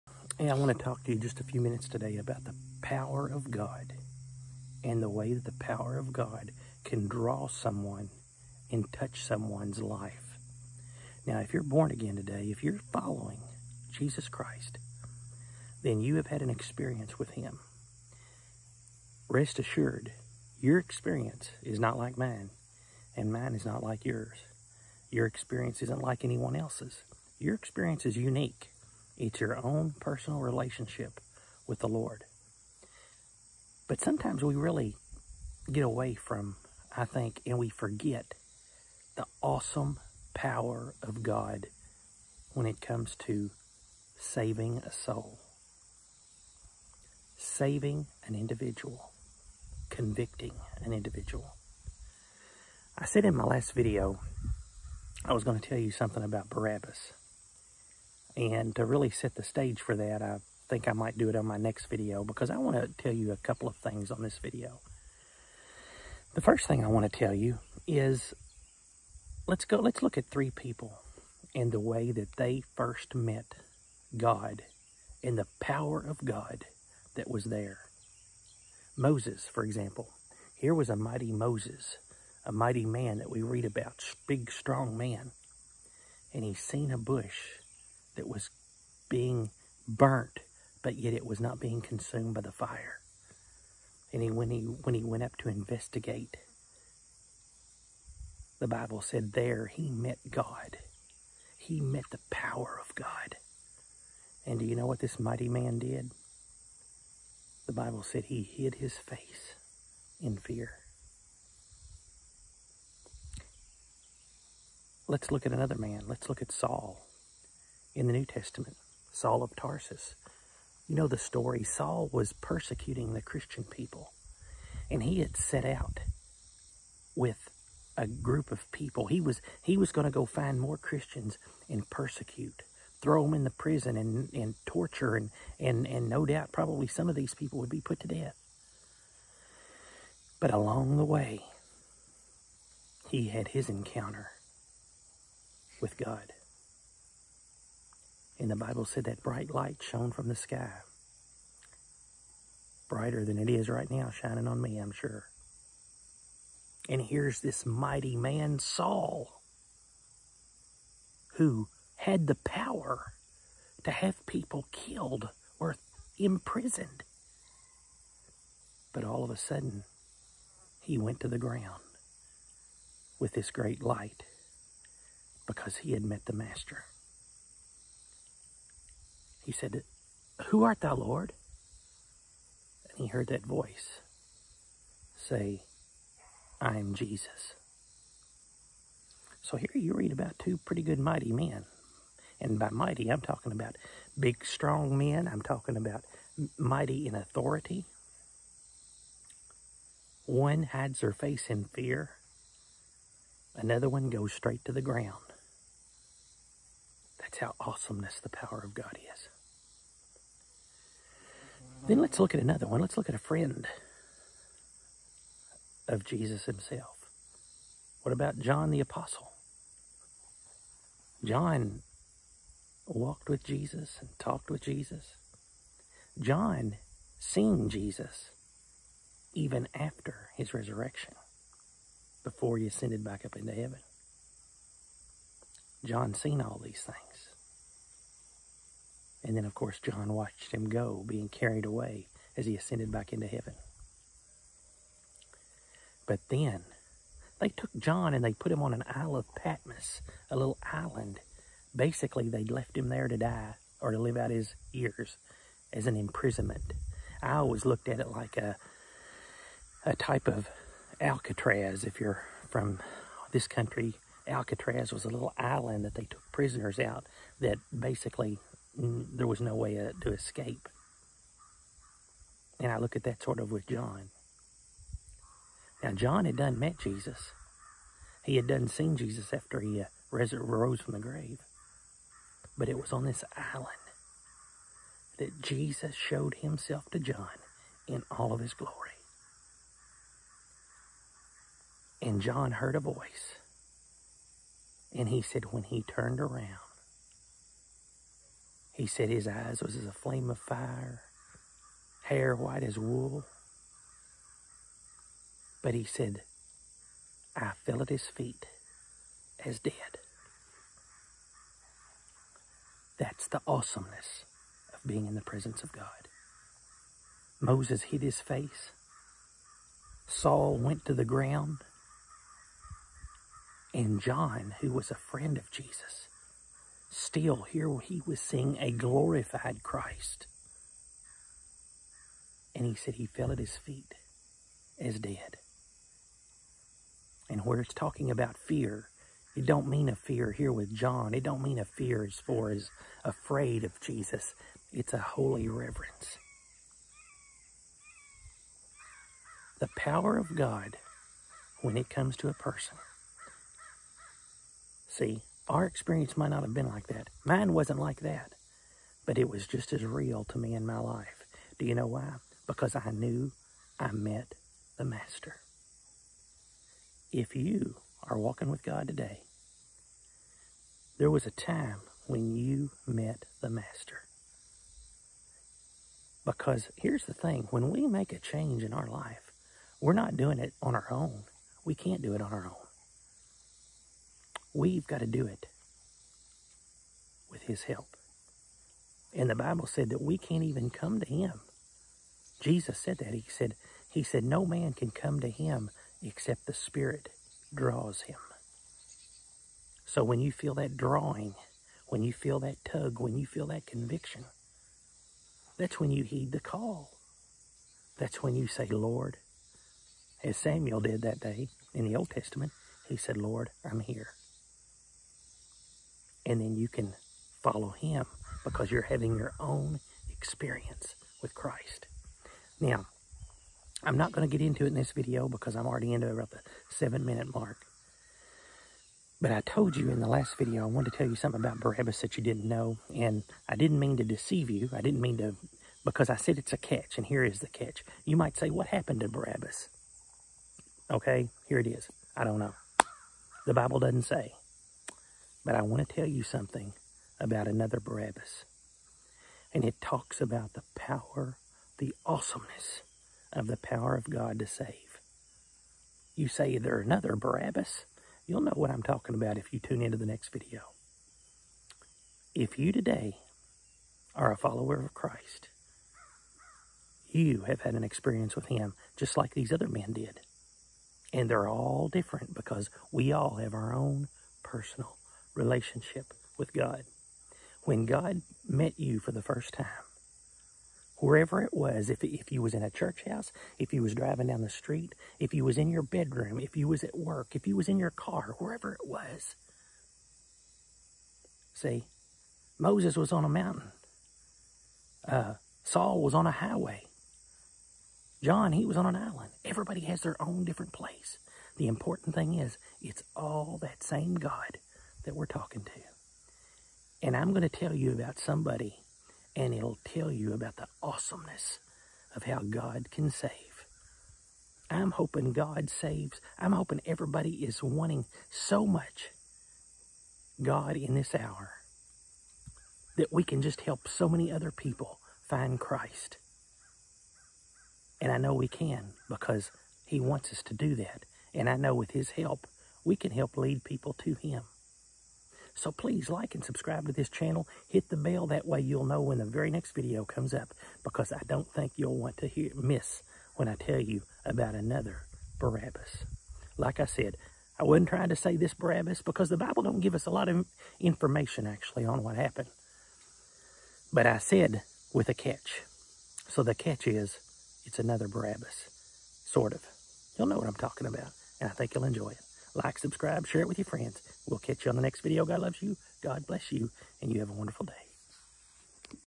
WHEN MAN MEETS THE MIGHTY PRESENCE OF GOD……if you’re saved, you’ve felt it...if not, YOU CAN
This video studies man and how God can make his presence felt in man’s life. We all have our own experience. If you have ever felt his draw or convicting power, you have had the same experience as some will look at. Not the same thing but it was a life changing experience nonetheless.

Verses referenced :
Exodus 3: 6
Revelation  1:17
Acts 9:4